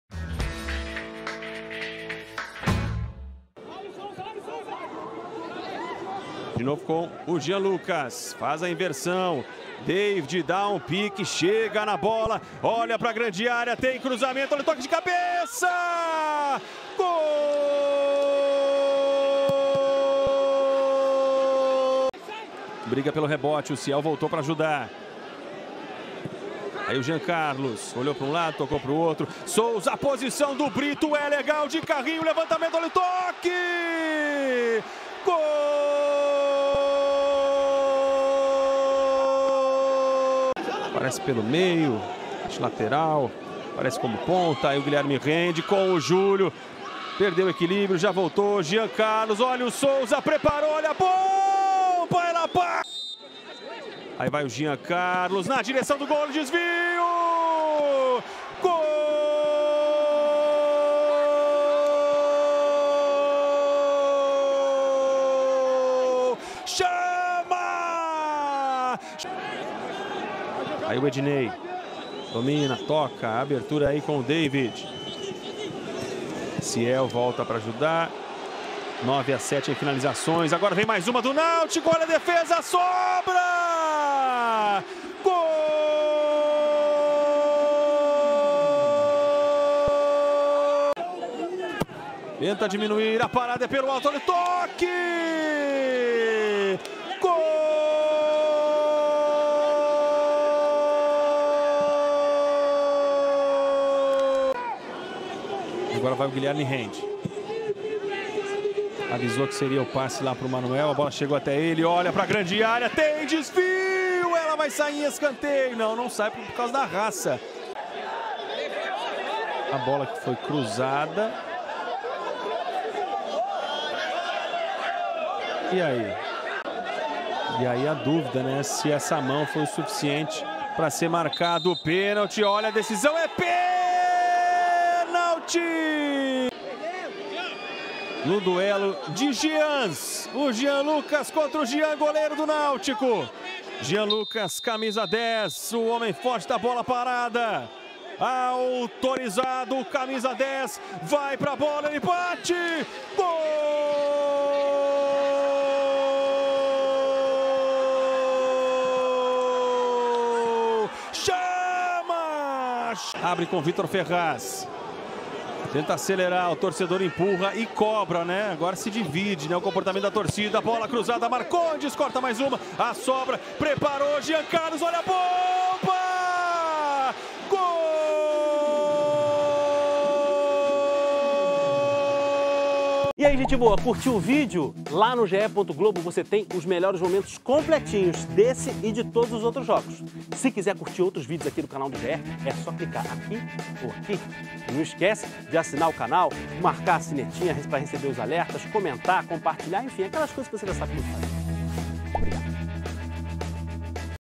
NÁUTICO 4 X 3 TOMBENSE | MELHORES MOMENTOS | 33ª RODADA BRASILEIRÃO SÉRIE B 2022 | ge.globo
Em jogo eletrizante, Náutico vence Tombense e segue vivo na Série B. Com o resultado, Timbu diminui distância para o primeiro time fora do Z-4 para sete pontos, restando cinco jogos para o final da competição; Carcará cai para o 12º lugar.